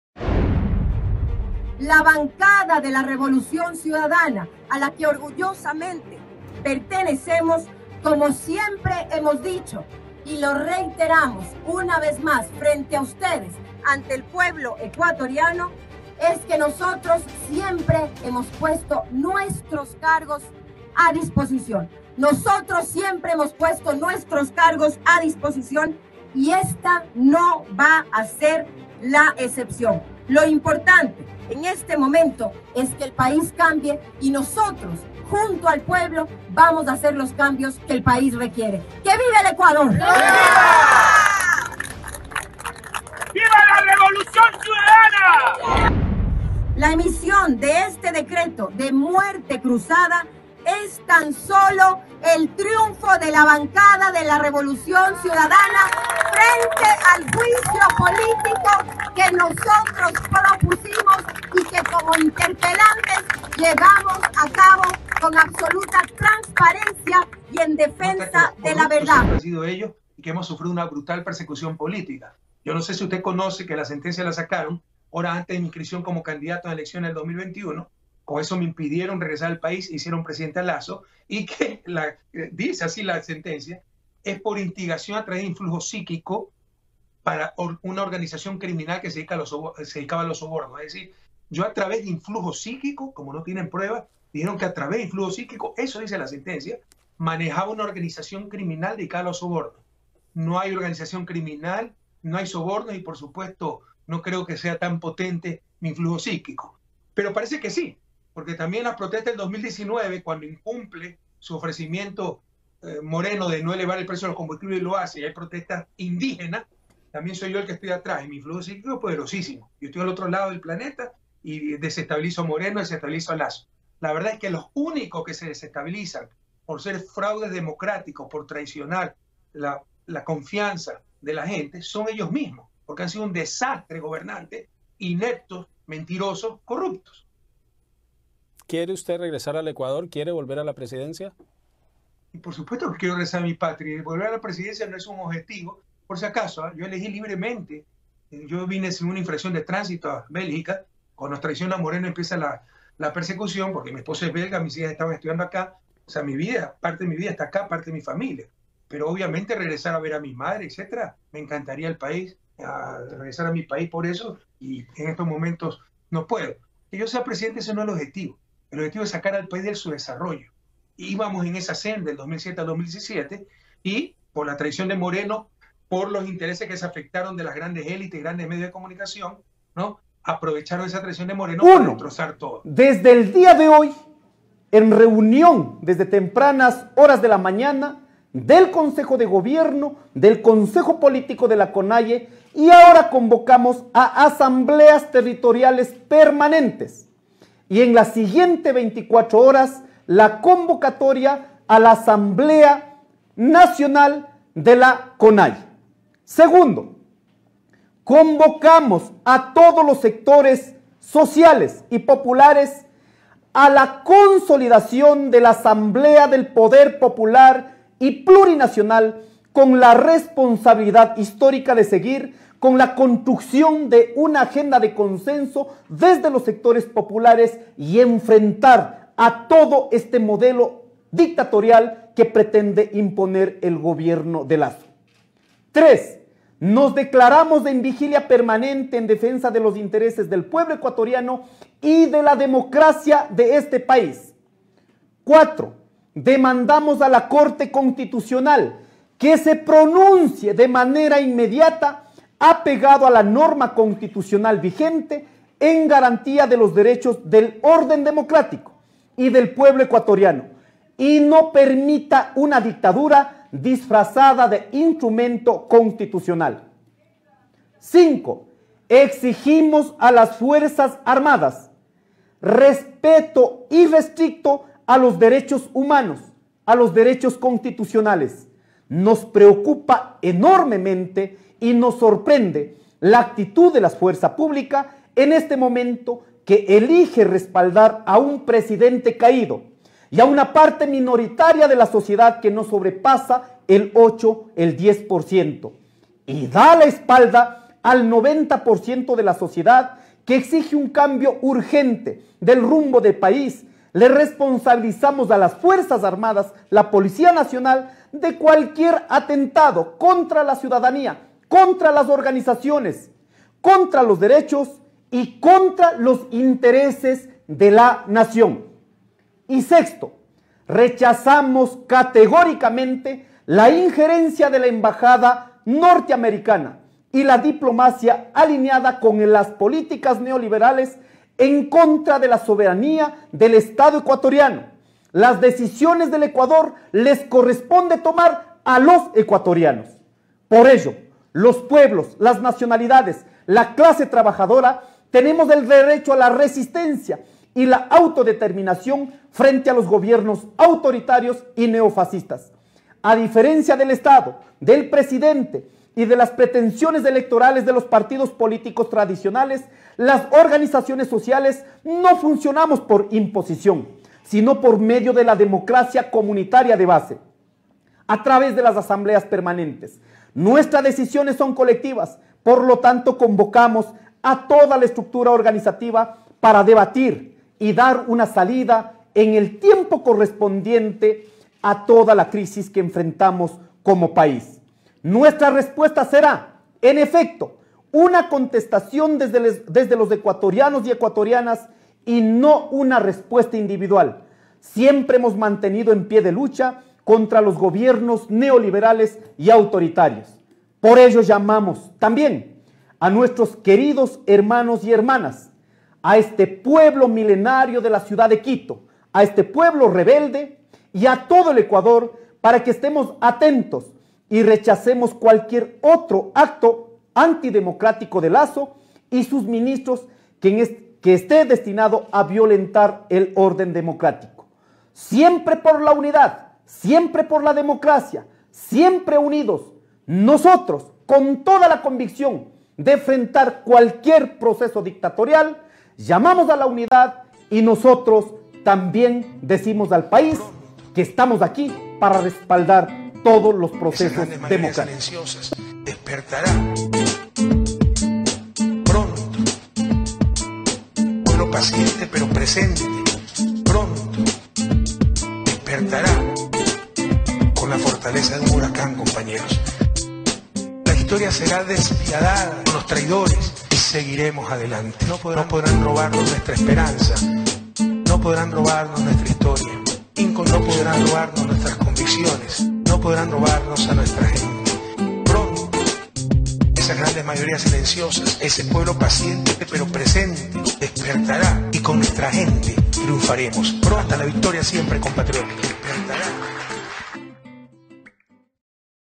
La Revolución Ciudadana se moviliza en el Ecuador frente a las medidas dictatoriales de Lasso
La Revolución Ciudadana se moviliza en el Ecuador frente a las medidas dictatoriales de Guillermo Lasso